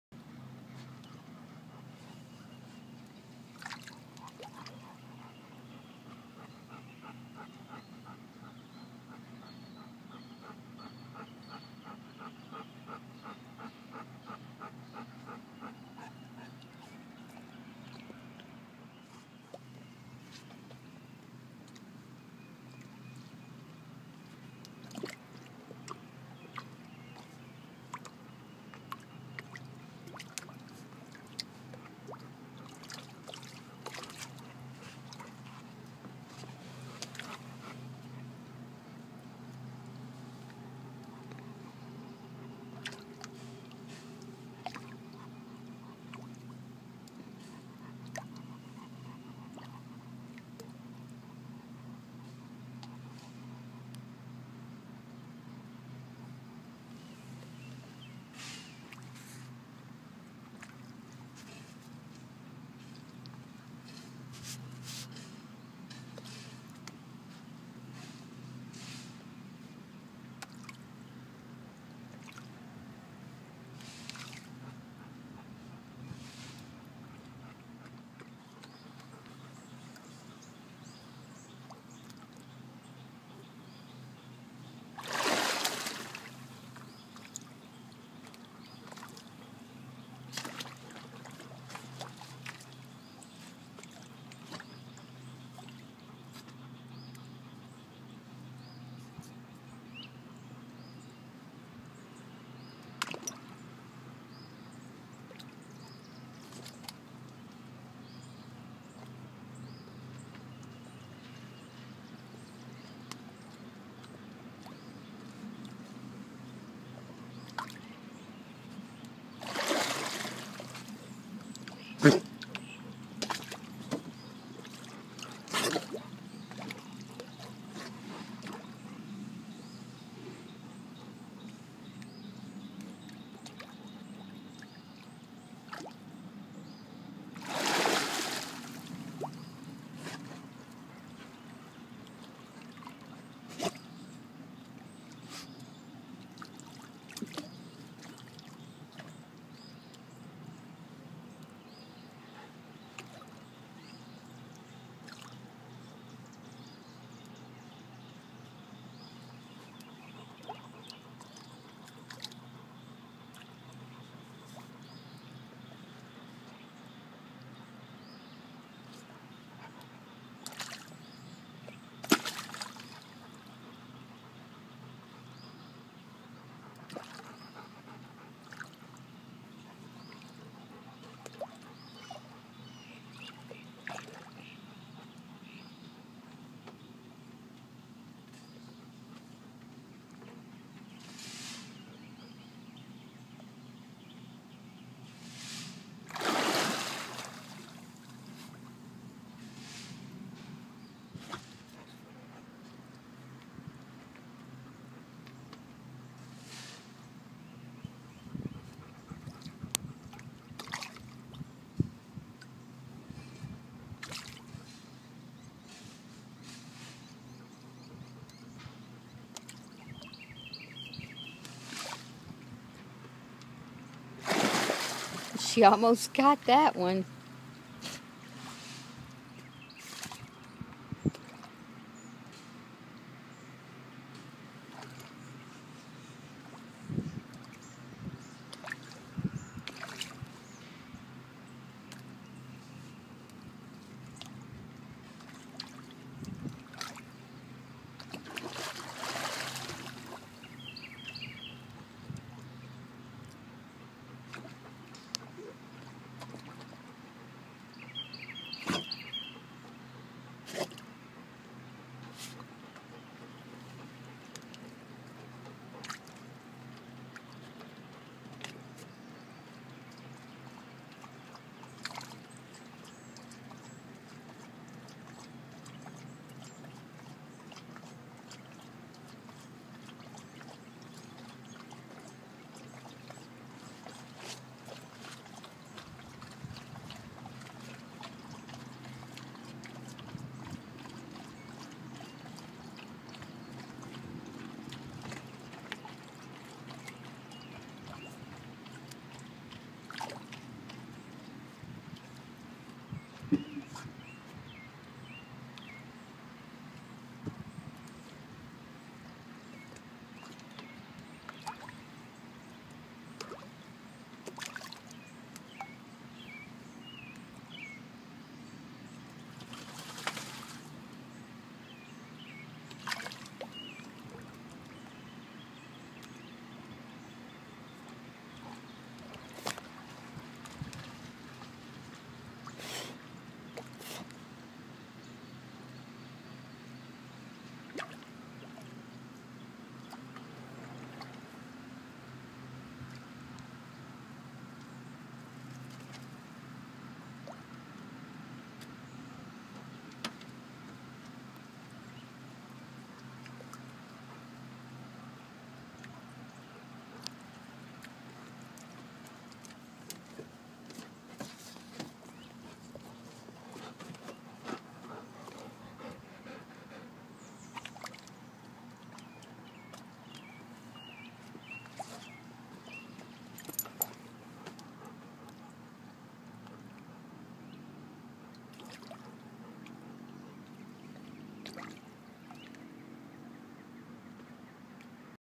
052414 Rani goes Fishing
Rani loves to fish off our pier on her little ramp,  She has caught 8 fish so far.  She caught a little bream this morning.  My husband threw it back in and a catfish ate it.